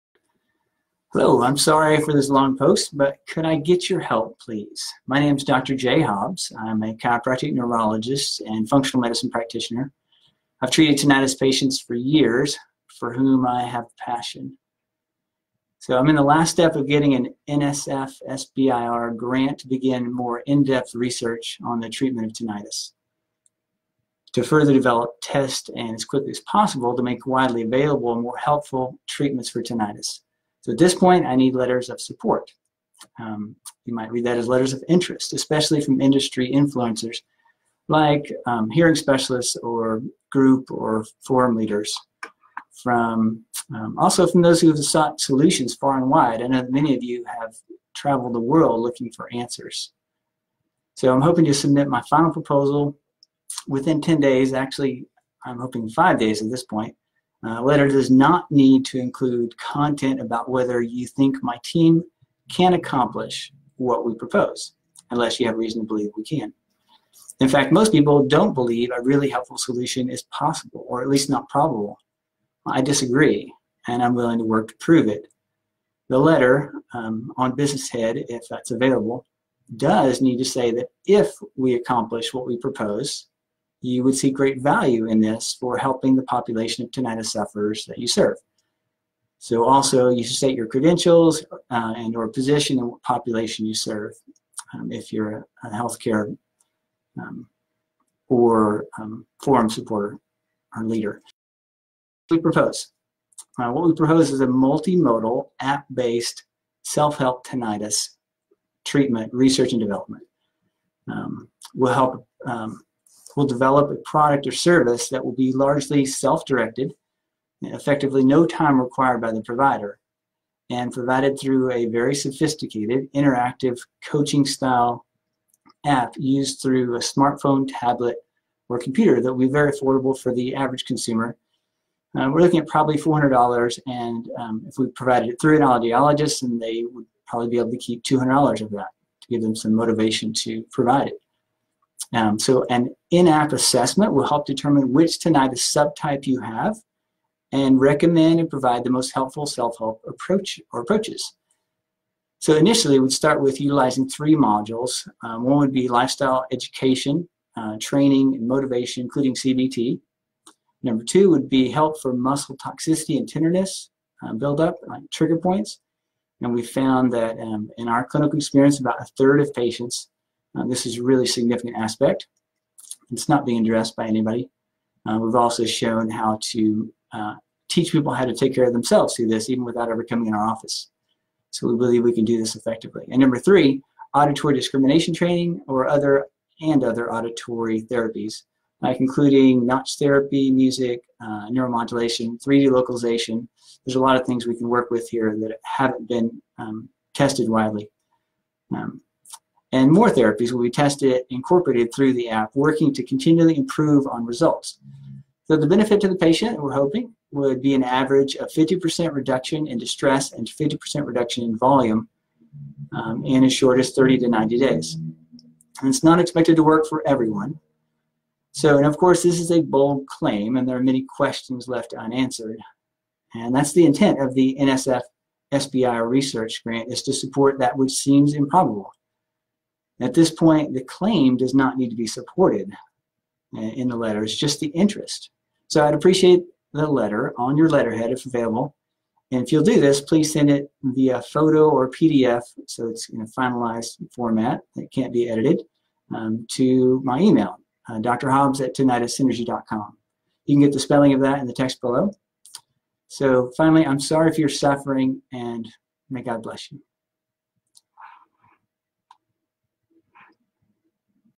Tinnitus Research - We need your help with support letters
Could I get your help?  My name is Dr. Jay Hobbs.  I am a chiropractic neurologist and functional medicine practitioner.  I have treated tinnitus patients for years, for whom I have  passion.  I am in the last step of getting a NSF-SBIR grant to begin more in-depth research on the treatment of tinnitus, to further develop, test and as quickly as possible, make widely available, more helpful treatments for tinnitus . At this point, I need "Letters of Support" (i.e. interest), especially from industry influencers, like hearing specialists or group or forum leaders, and from those who have sought solutions far and wide. I am hoping to submit my final proposal within 10 (actually only 5 now) days. The letter does NOT need to include content about whether you think my team can accomplish what we propose (unless you have reason to believe we can). In fact, most people do not believe a really helpful solution is possible, or at least not probable.  I disagree, and I’m willing to work to prove it.  The letter (on business letterhead, if available) DOES need to say that IF we accomplish what we propose, you would see great value in this for helping the population of tinnitus sufferers you serve. Also, you should state your credentials/position and what population you serve.
What we propose: Multi-modal, app-based, self-help tinnitus treatment R & D: We will develop a product/service that will be largely self-directed (effectively no time required by the provider) and provided through a very sophisticated(interactive, coaching-style) app used through a smartphone, tablet, or computer that will be very affordable for the average consumer (~$400, and the provider would keep $200). An in-app assessment will help determine which tinnitus subtype you have and recommend and provide the most helpful self-help approach(es). Initially it will utilize 3 modules: 1) lifestyle education, training, and motivation, including CBT, 2) help for muscle toxicity/tenderness build-up (trigger points), and 3) auditory discrimination training and other auditory therapies (incl notched music, neuromodulation, 3D localization, etc). More therapies will be tested and incorporated through the app, working to continually improve on results. The benefit to the patient/client will be an average of 50% reduction in distress and 50% reduction in volume over 30-90 days.  It is not expected to work for everyone.
Of course, that is a bold claim, and there are many questions left unanswered. The intent of NSF-SBIR research is to support that which seems improbable. At this point, the CLAIM does not need to be supported, just the INTEREST. I would appreciate the letter, on your letterhead, if available.
I am sorry for your suffering.  May God bless you.